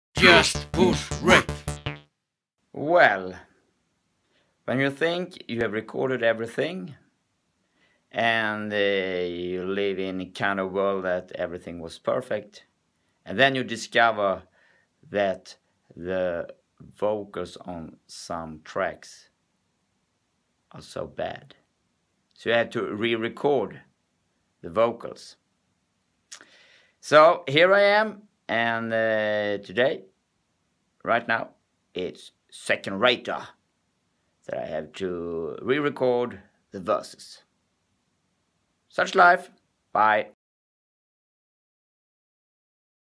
18. Re-re-recording
When a recording isn’t up to par with what is required, it is only to bite the bullet and re-re-re-re-re-record, or, the song won’t make it on the record. But now, all done in record time.